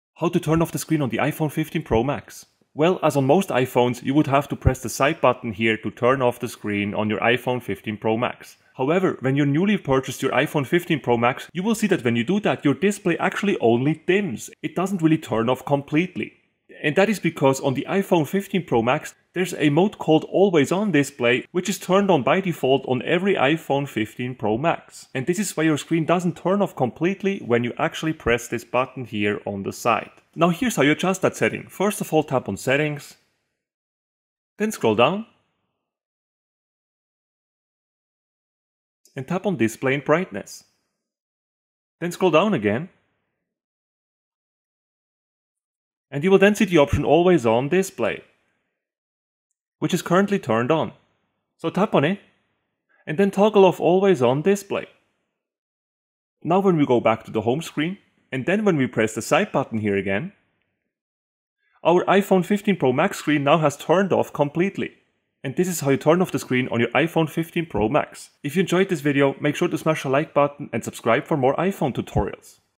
iPhone 15 Pro Max How to Turn Off Screen — Beginner's Guide
In this video, you will learn how to turn off the screen completely on your iPhone 15 Pro Max phone. It's completely normal that your screen only dims when you first purchase the iPhone 15 Pro Max. But it might confuse you because with older iPhones, this was actually never the case. But don't worry, it's just a new default setting that we will need to adjust to make this work as "expected" again. I will walk you through all the necessary steps in this video to make sure your iPhone 15 screen turns off completely whenever you want it to do so!